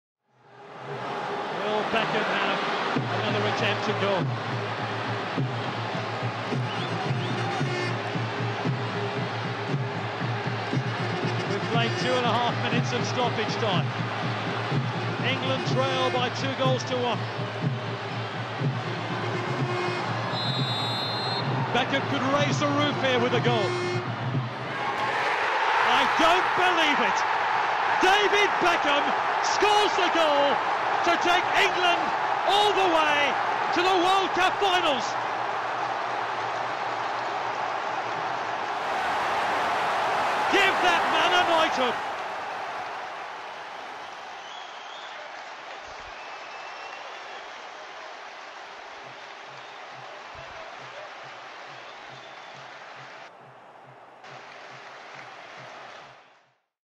El MEJOR GOL de David Beckham con Inglaterra | Golzo de FALTA Inglaterra - Grecia
David Beckham, uno de los mejores lanzadores de falta de la historia. Hoy recordamos uno de los libres directos más increíbles de su carrera.
En el último partido de la fase de clasificación para el mundial de Corea y Japón 2002, Inglaterra necesitaba un empate y sucedía esto...